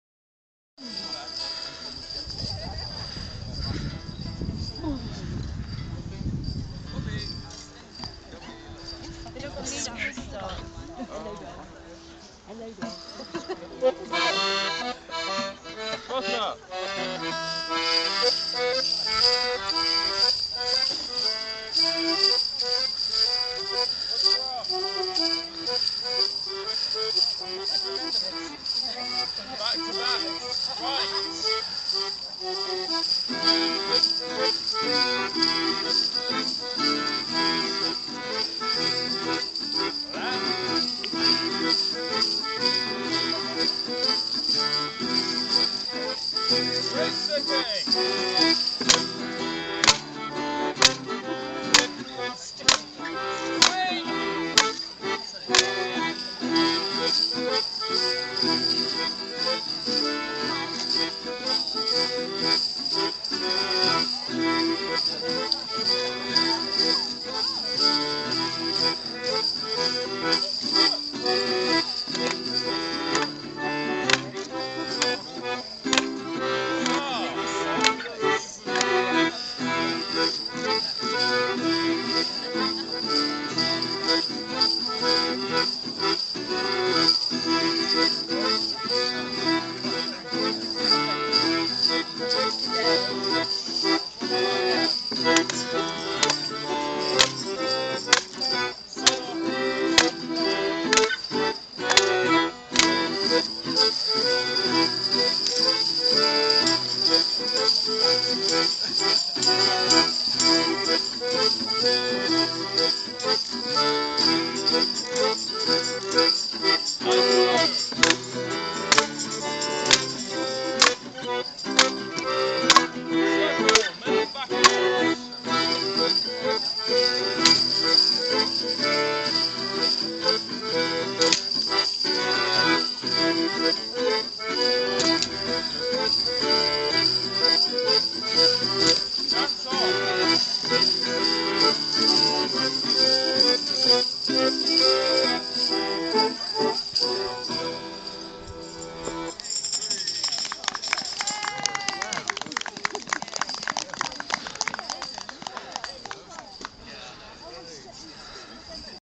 May Day Morris 2013 at the Black Mill pt 3
Traditional sunrise (5:25am) May Day morning dance out at the Black Mill, Beverley Westwood. Taking the reins from the now retired Greenginger Morris team who had maintained the tradition for the last 15 years, dances performed by the Rackaback Morris and of course the spectating public.